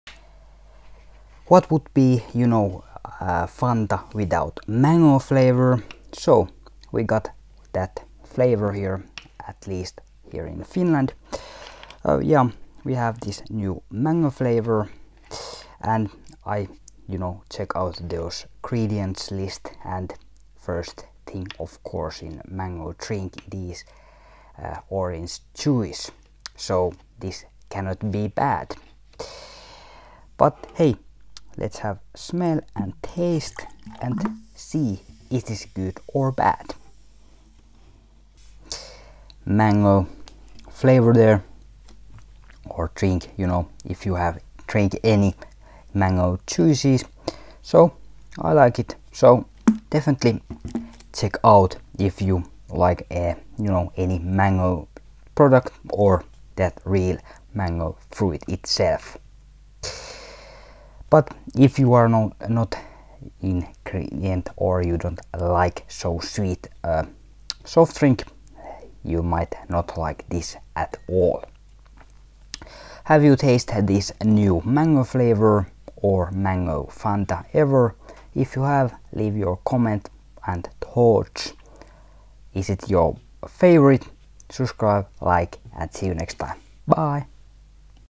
Where is the Mango ?
Taste test of New Fanta flavour Mango.